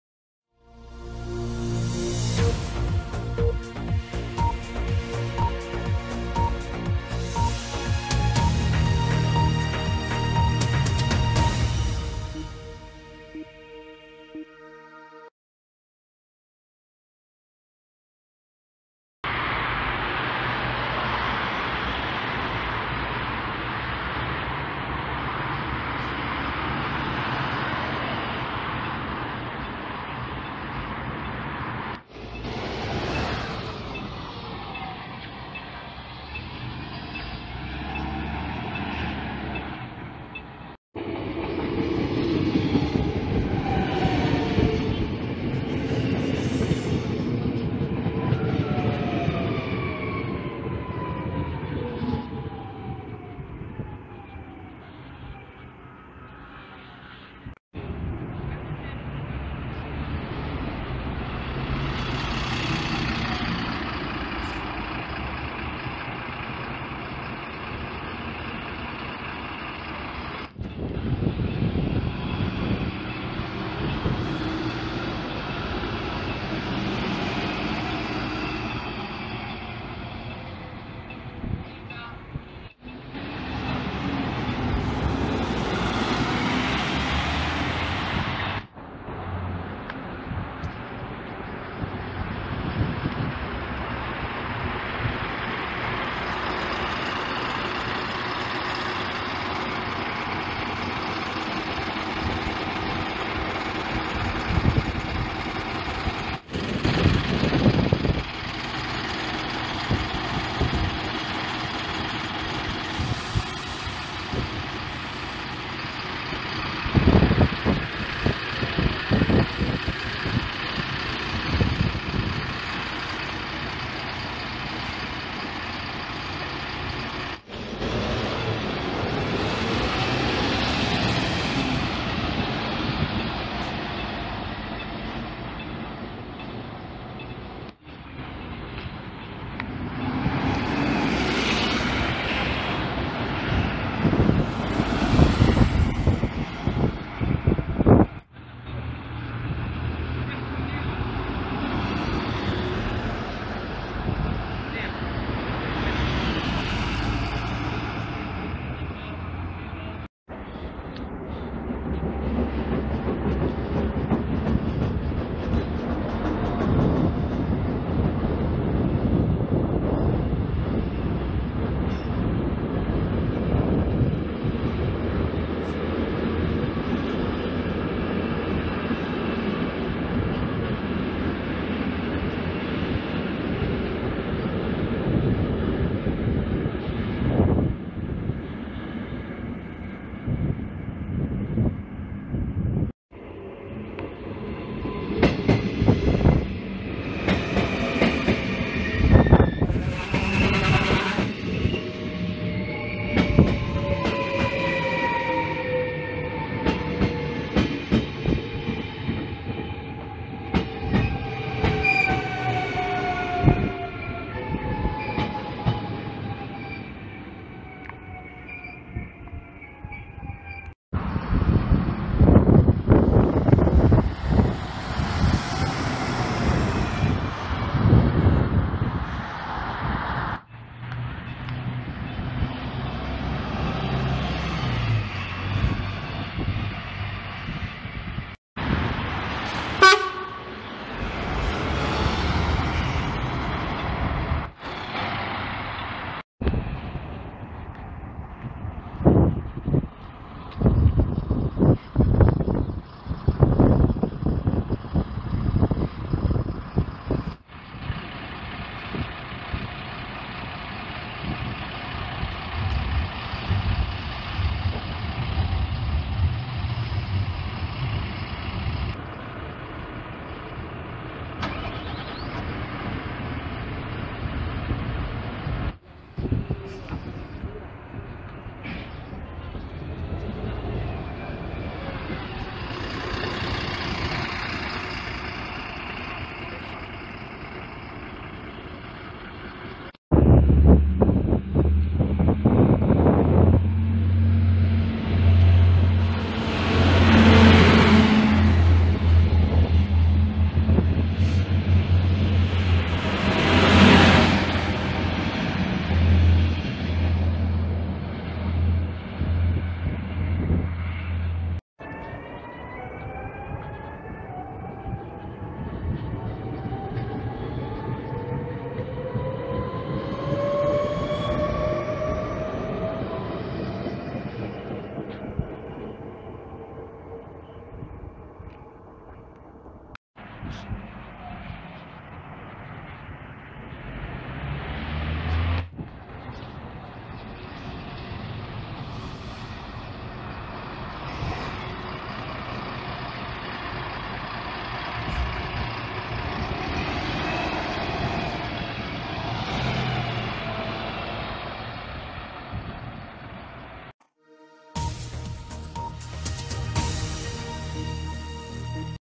Nieuwe Bussen en Treinen in Roermond 23-03-2017
Als laatste ben ik naar Roermond geweest.
Het busstation van Roermond is best klein.
Hier rijden de VDL Citea en de Volvo 8900.
De meeste bussen rijden 2x per uur.en vertrekken ook tegelijk.
Het stationsgebouw is wel heer erg mooi.
maar op het station zelf is niet echt veel te beleven.
De Volvo,s vind ik trouwens de mooiste bus.

Veel kijkplezier.